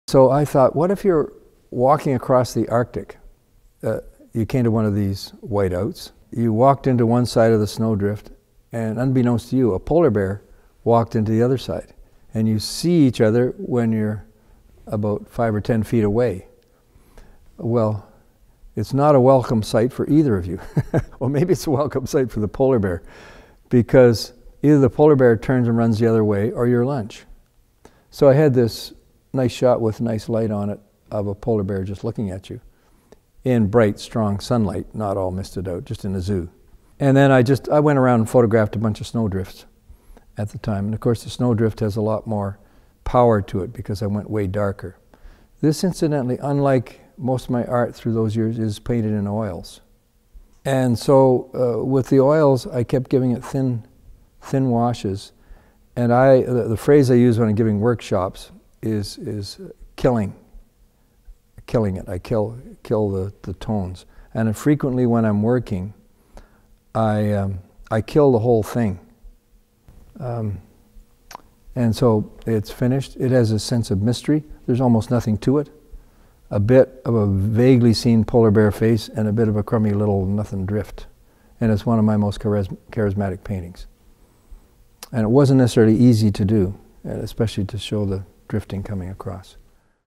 #05 White Encounter by Robert Bateman
White Encounter by Robert Bateman
1980, 36" x 48", Acrylic

"What if, traveling the Arctic, you entered a blowing snow drift and, unknown to you, a polar bear entered from the other side! By the time you both knew this, it would be too late. This painting is the way I expressed the discovery."

Robert Bateman discusses his behind the scenes process of some of his most iconic paintings. To view the largest permanent collection of his work in the world spanning seven decades, come visit the Robert Bateman Centre in Victoria BC or view his entire collection